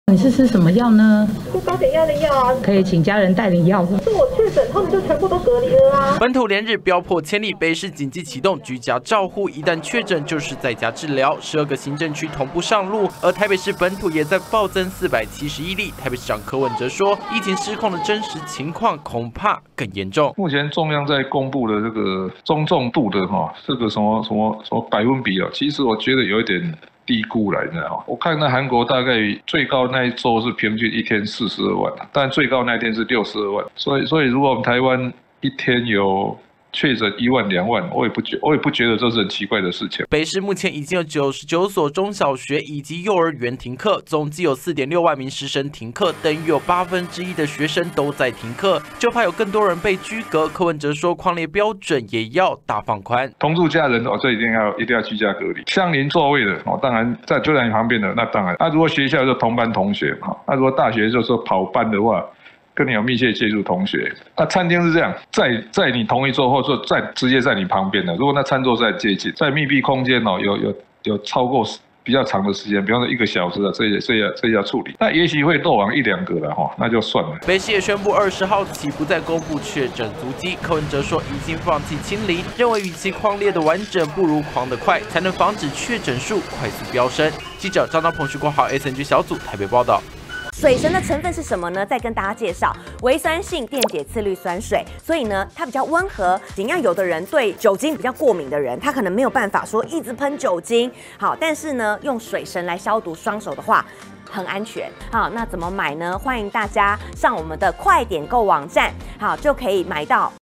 【每日必看】疫情比看到的嚴重? 台北不公布足跡 放寬匡列標準@中天新聞CtiNews @毛球烏托邦MaoUtopia 20220420
本土確診連續數天飆破千例,但台北市長柯文哲說,恐怖的根本還沒到來,韓國最高紀錄一天就有42萬例,台灣未來每天一兩萬例也不奇怪,由於目前確診人數太多，足跡也太多!同時柯文哲也宣布從20日起 ,不再公布足跡,還要放寬匡列標準。